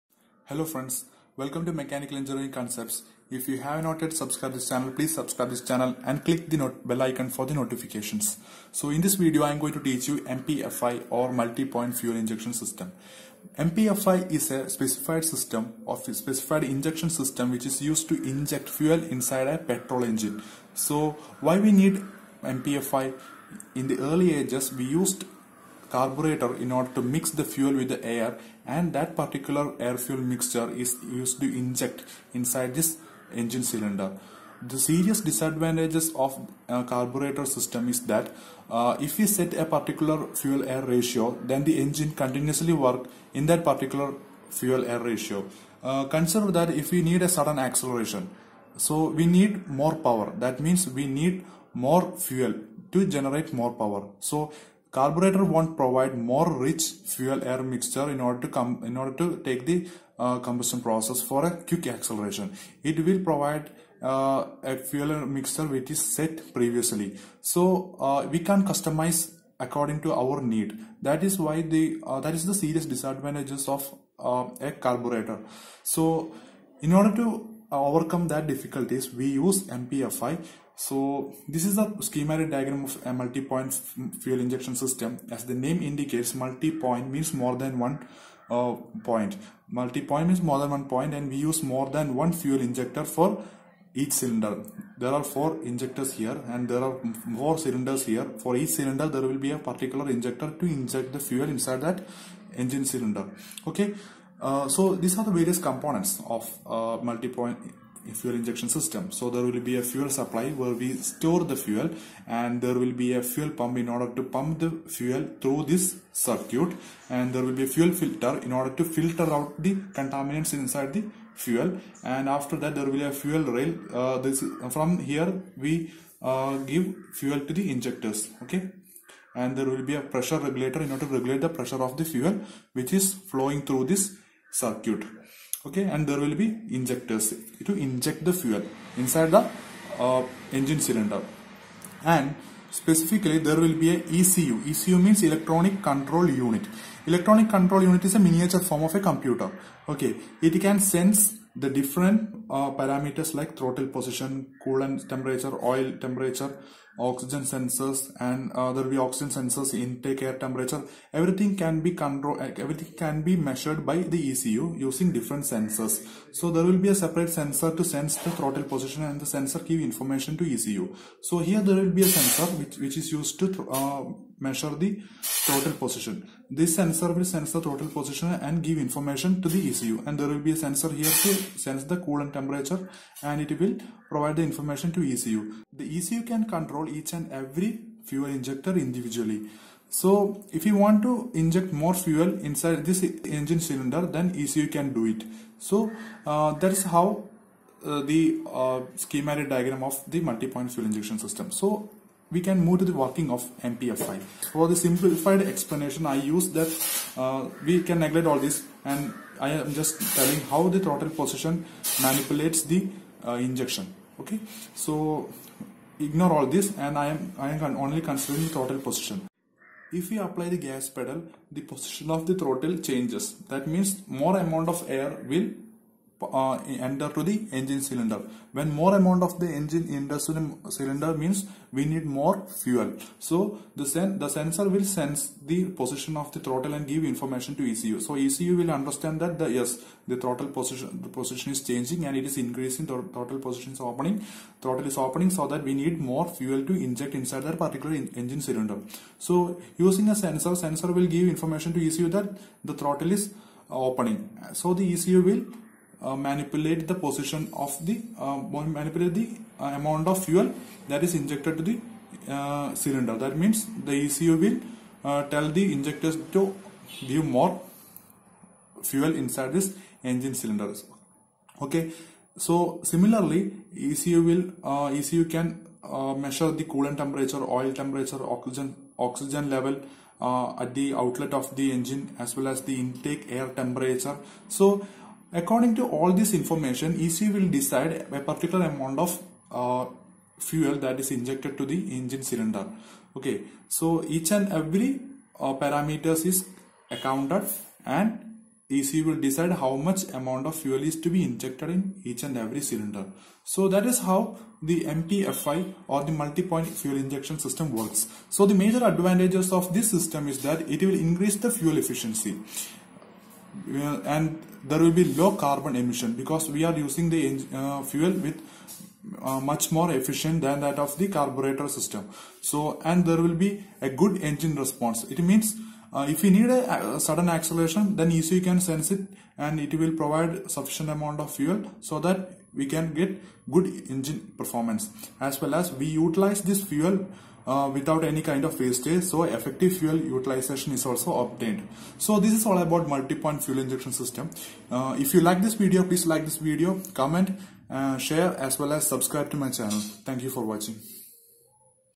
MPFI-Multi Point Fuel Injection System Explained
The MPFI or multi point fuel injection system is a system or method of injecting fuel into internal combustion engine through multi ports situated on intake valve of each cylinder. It delivers an exact quantity of fuel in each cylinder at the right time.